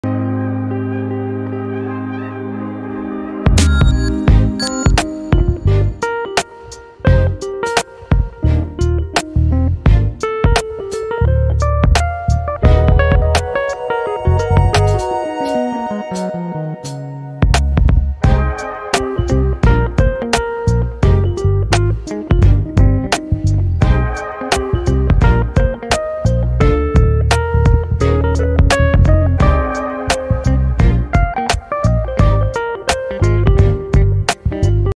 8 Marla house for Sale in , Islamabad
8 Marla house for Sale in , Islamabad

 Bathrooms: 7
 Bedrooms: 4
 Stories: 2
 Demand: Rs.23,000,000
 Electricity: Available
 Water: Supply Water
 Gas:

 Location: ,Islamabad